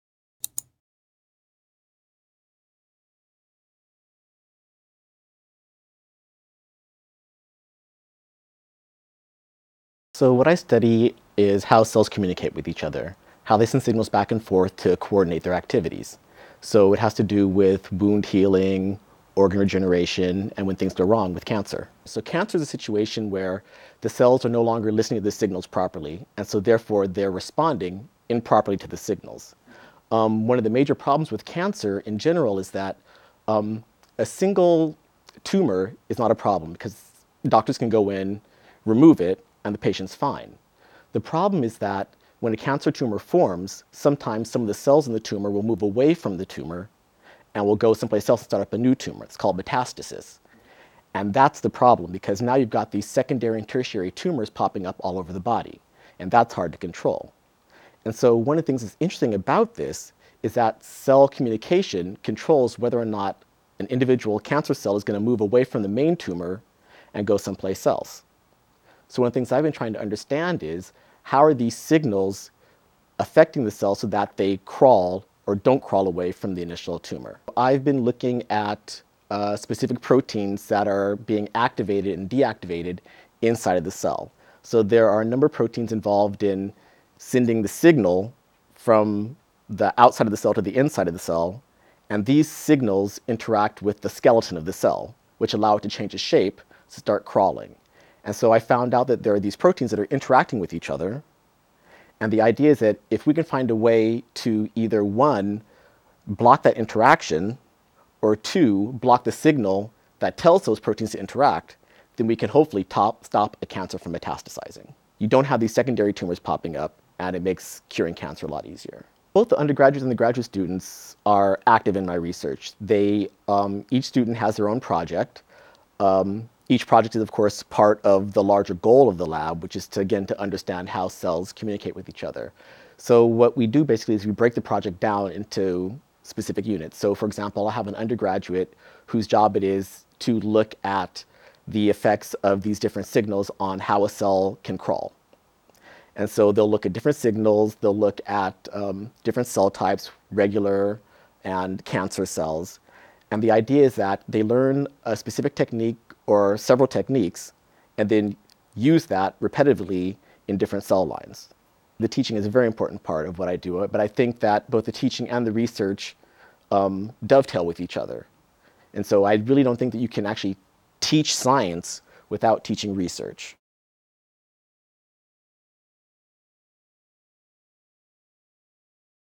Cell Communication
Hunter College biologist Derrick Brazill says the cure for cancer begins with understanding how cells communicate with each other.